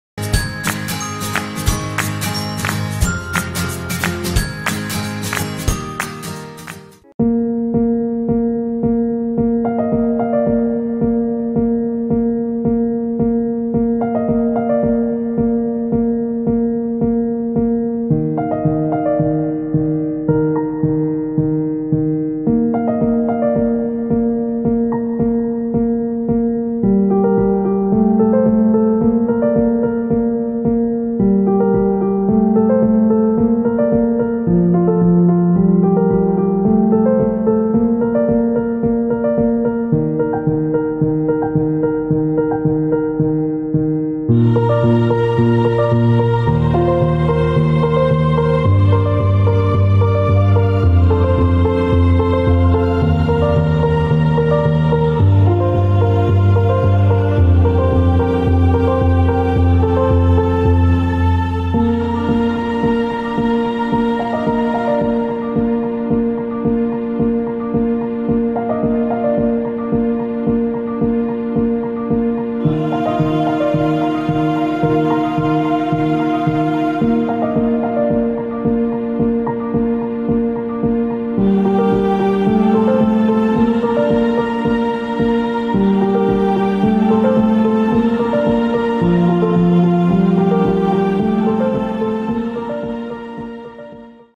Lavazza Point Espresso Coffee Filling Sealing Machine
Automatic Lavazza Point Coffee Capsule Filling Sealing Machine with Vibration empty capsule feeding, vacuum powder feeding, UV sterilization, Anti-static, perforated film cutting sealing, Twice Sealing, Inline weight checking, output conveyor..